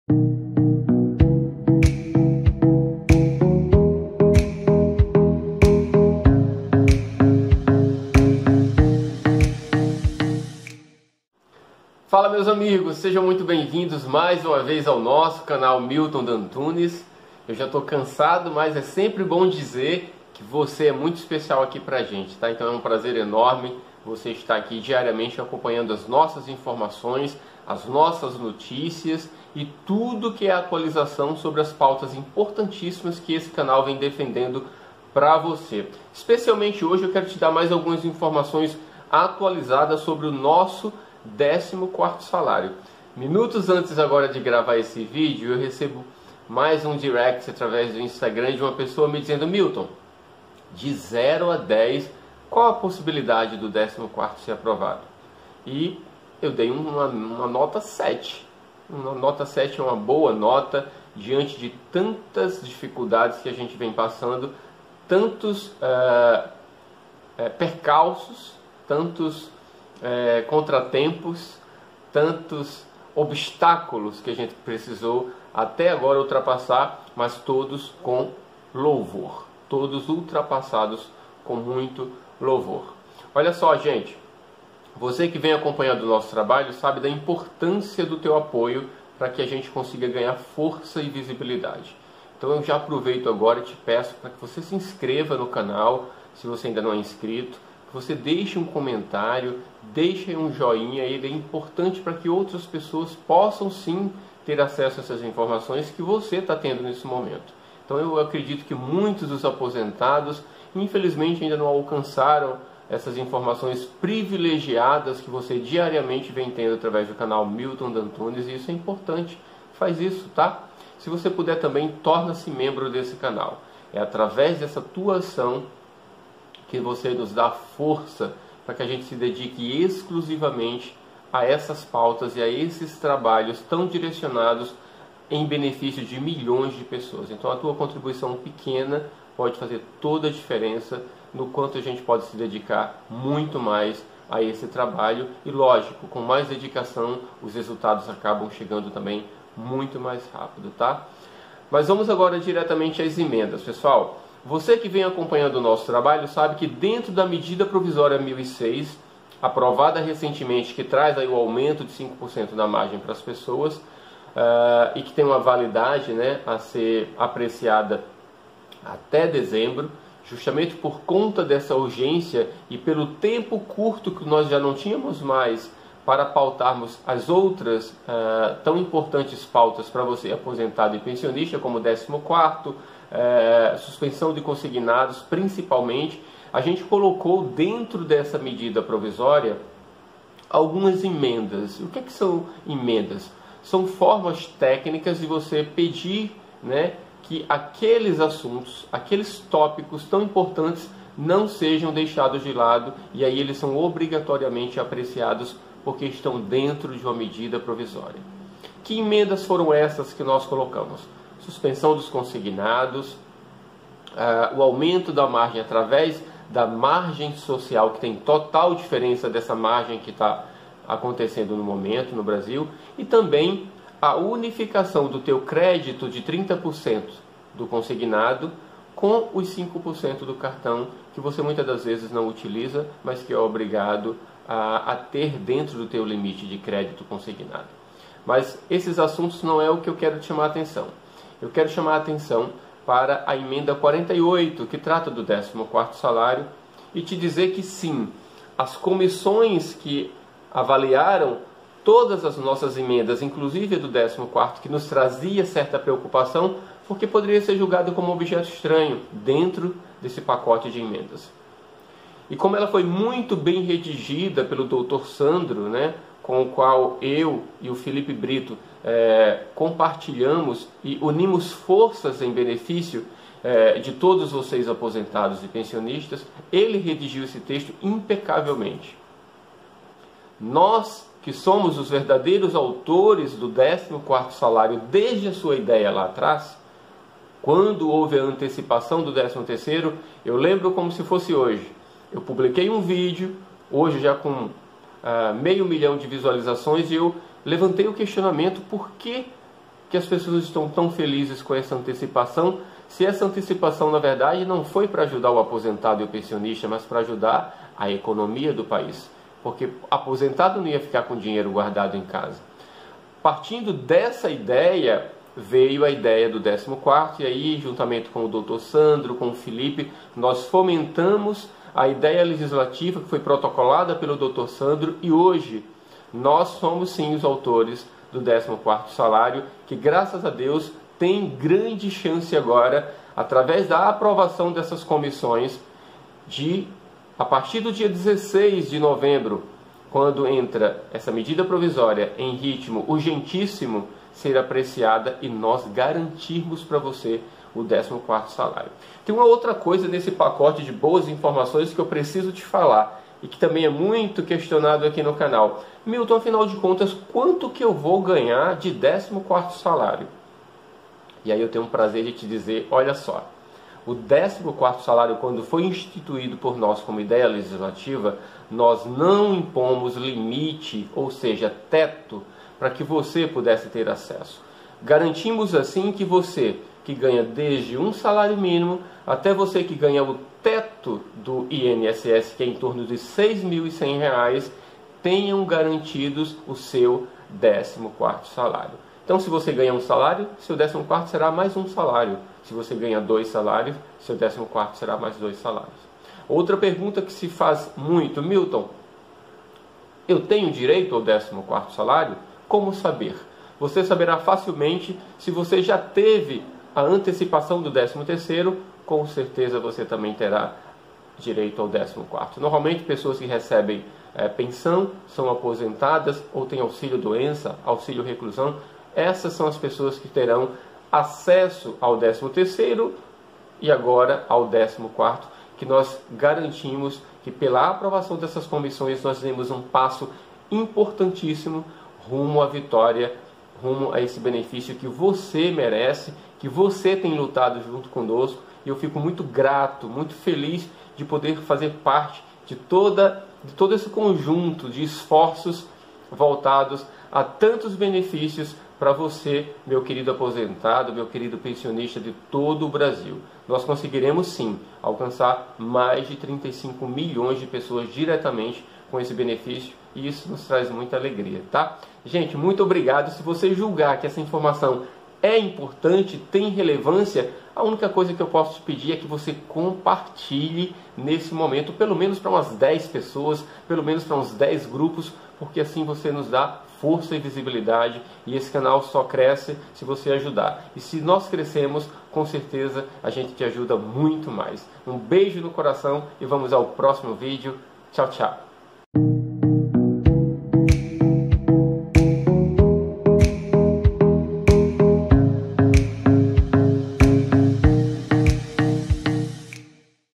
14º SALÁRIO APROVADO NAS COMISSÕES! Valores do Benefício Podem Ser de Até 6 Mil Reais!
14º SALÁRIO APROVADO NAS COMISSÕES! Valores do Benefício Podem Ser de Até 6 Mil Reais!

As comissões que analisaram todas as emendas contidas na MP 1006 foram aprovadas entre estas encontra-se a emenda 48 que trata do décimo quarto salário para o ano de 2020 aos beneficiário do inss, serão cerca de 35 milhões de pessoas diretamente beneficiadas.
Os valores do décimo quarto salário aprovado nas comissões poderá chegar até 6 mil reais já que não foi estipulado teto máximo para pagamento.

PRECISAMOS DO SEU APOIO PARA O DÉCIO TERCEIRO PRO PBC
( BEBEFÍCIO DE PRESTAÇÃO CONTINUADA )

  @Milton Dantunes